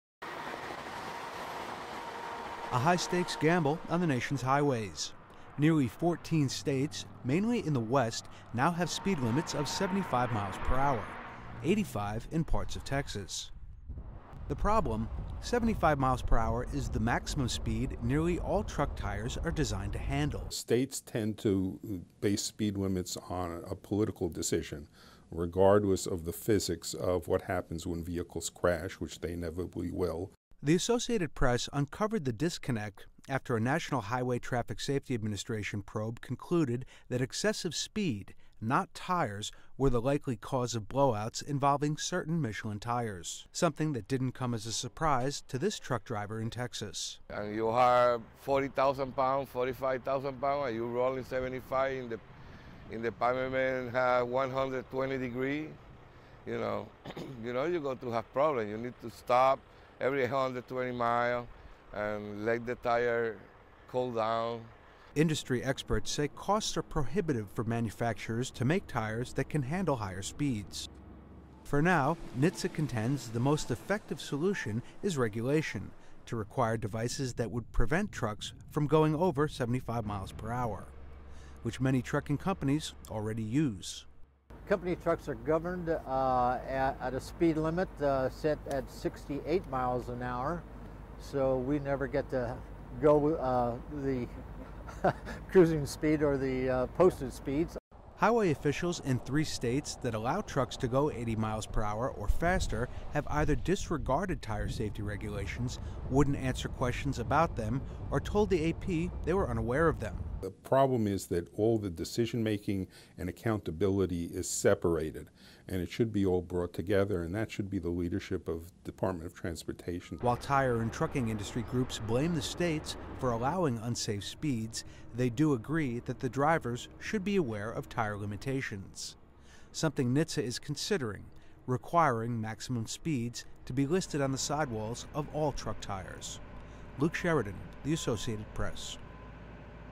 Many tractor-trailers on the nation's roads are driven faster than the 75 mph their tires are design
(31 Mar 2015) VOICE-OVER:
A HIGH STAKES GAMBLE ON THE NATION'S HIGHWAYS.
NEARLY 14 STATES, MAINLY IN THE WEST NOW HAVE SPEED LIMITS OF 75... 85 IN PARTS OF TEXAS.
THE PROBLEM?
75 MPH IS THE MAXIMUM SPEED NEARLY ALL TRUCK TIRES ARE DESIGNED TO HANDLE.
SOUNDBITE (English) Henry Jasny, Senior VP, Advocates for Highway and Auto Safety:
"States tend to base speed limits on a political decision, regardless of the physics of what happens when vehicles crash, which they inevitably will."
THE ASSOCIATED PRESS UNCOVERED THE DISCONNECT AFTER A NATIONAL HIGHWAY TRAFFIC SAFETY ADMINISTRATION PROBE CONCLUDED THAT EXCESSIVE SPEED - NOT TIRES - WERE THE LIKELY CAUSE OF BLOWOUTS INVOLVING CERTAIN MICHELIN TIRES.
SOMETHING THAT DIDN'T COME AS A SURPRISE TO THIS TRUCK DRIVER IN TEXAS.
SOUNDBITE (English) David Ortiz, truck driver
"You have 40,000 pounds or 45,000 pounds and you are rolling at 75 mph and the pavement is 120 degrees, you know you're going to have problems. You need to stop every 120 miles and let the tires cool down."
INDUSTRY EXPERTS SAY COSTS ARE PROHIBITIVE FOR MANUFACTURERS TO MAKE TIRES THAT CAN HANDLE HIGHER SPEEDS.
FOR NOW... NHTSA CONTENDS THE MOST EFFECTIVE SOLUTION IS REGULATION TO REQUIRE DEVICES THAT WOULD PREVENT TRUCKS FROM GOING OVER 75 MPH.
WHICH MANY TRUCKING COMPANIES ALREADY USE.
SOUNDBITE (English) Steve Valdespino, truck driver:
"Company trucks are governed at a speed of 68 mph, so we never get to go over the cruising speed, or posted speeds."
HIGHWAY OFFICIALS IN THREE STATES THAT ALLOW TRUCKS TO GO 80 MPH OR MORE HAVE EITHER DISREGARDED TIRE SAFETY RATINGS, WOULDN'T ANSWER QUESTIONS ABOUT THEM OR TOLD THE AP THEY WERE UNAWARE OF THEM.
"The problem is that all the decision making and accountability is separated and it should all be brought together, and that should be the leadership of the Department of Transportation."
WHILE TIRE AND TRUCKING INDUSTRY GROUPS BLAME THE STATES FOR ALLOWING UNSAFE SPEEDS, THEY DO AGREE THAT THE DRIVERS SHOULD BE AWARE OF TIRE LIMITATIONS.
SOMETHING NHTSA  IS CONSIDERING - REQUIRING MAXIMUM SPEEDS BE LISTED ON THE SIDEWALLS OF ALL TRUCK TIRES.
LUKE SHERIDAN -- ASSOCIATED PRESS